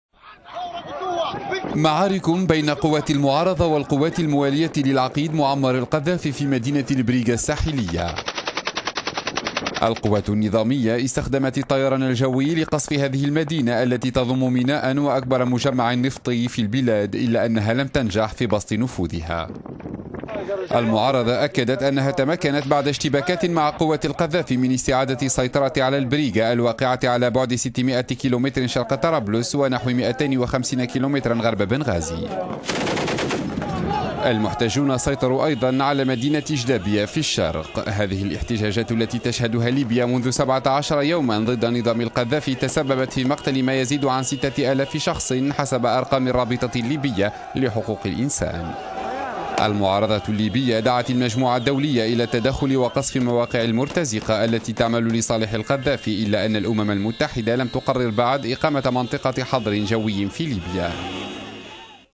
القوات الموالية للقذافي تفشل...
معارك بين قوات المعارضة والقوات الموالية للعقيد معمر القذافي في مدينة البريقة الساحلية. القوات النظامية إستخدمت الطيران الجوي لقصف هذه المدينة التي تضم ميناء وأكبر مجمع نفطي في البلاد، إلاّ أنها لم تنجح في بسط نفوذها.